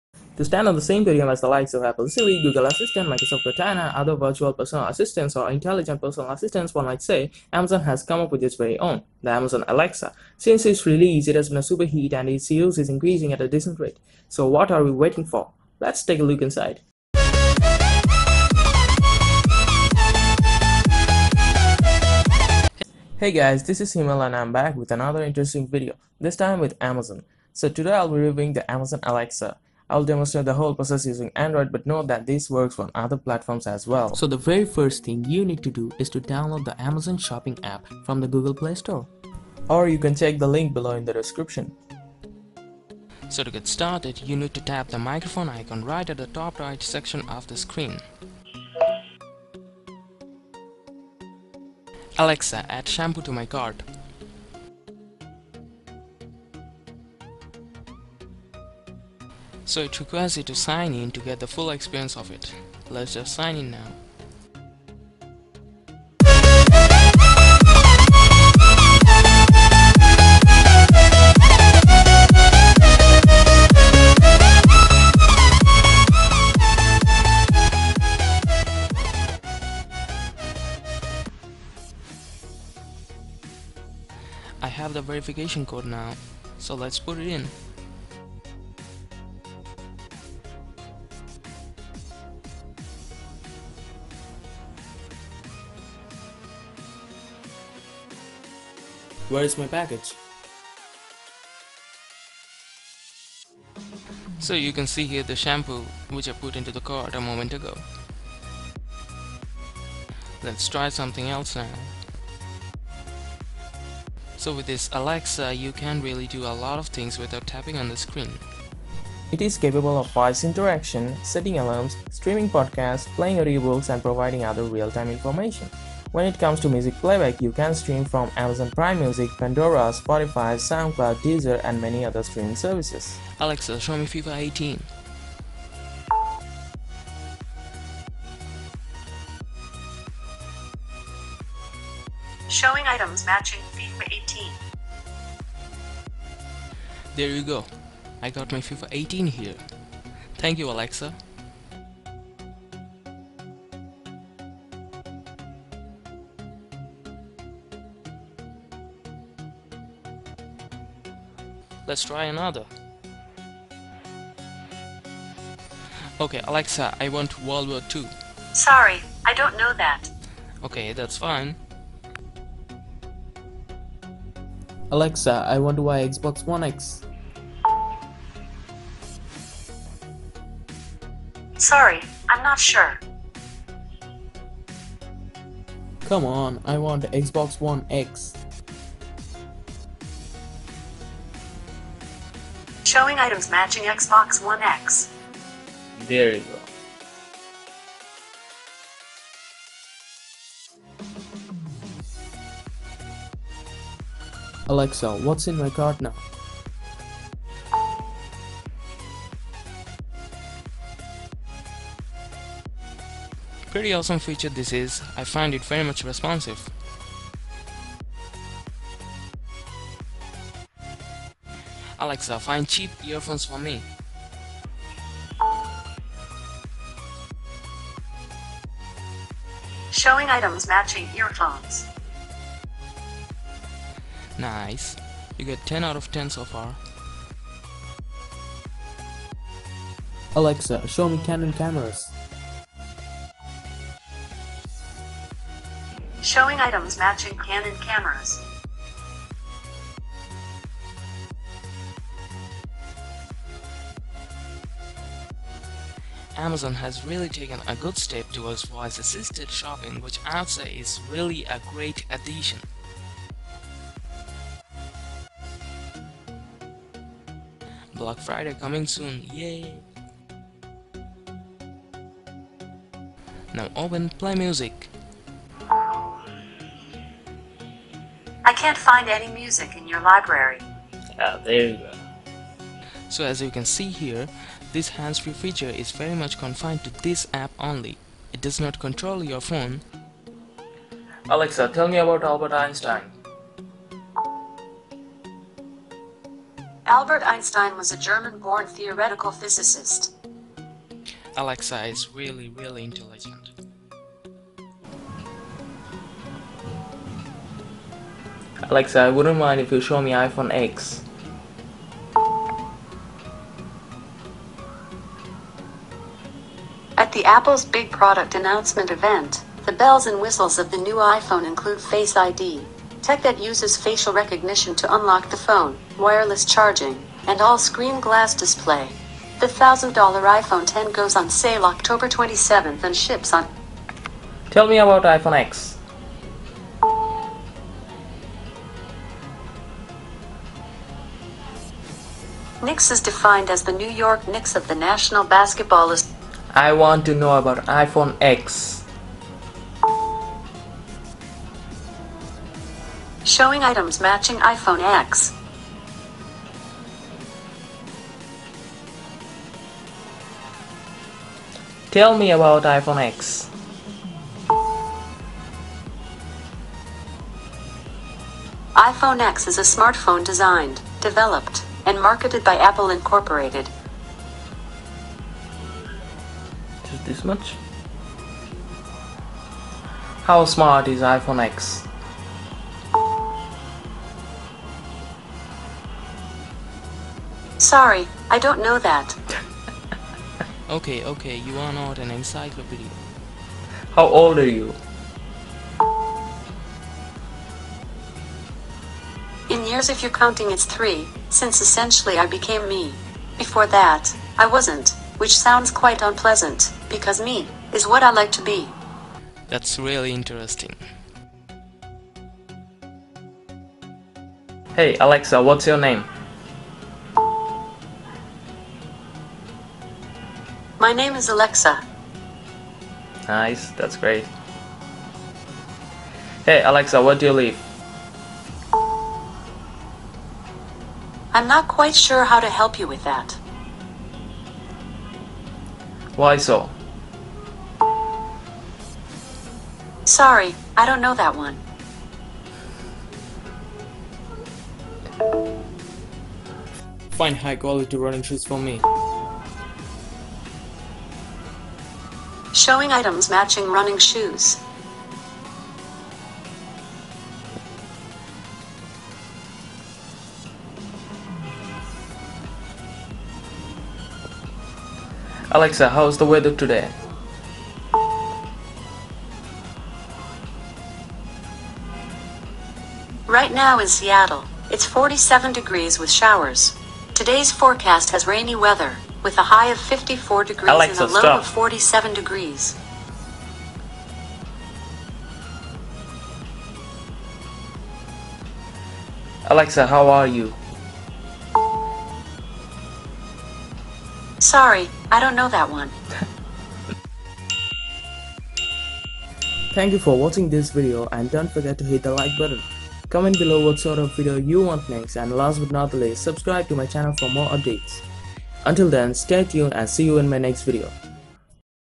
Amazon Alexa ● Personal voice assistant on the go [Android, iOS, Fire OS]
Artificial Intelligence (AI) is taking over the most innovative technologies everywhere, offering the voice interaction as one of its feature. En route to its integration to the cutting-edge technologies, big names have already shifted their focus on this new addition. With Apple Siri, Google Assistant, Microsoft Cortana already thriving on the way, Amazon introduces it own 'Alexa' to vie for the top spot.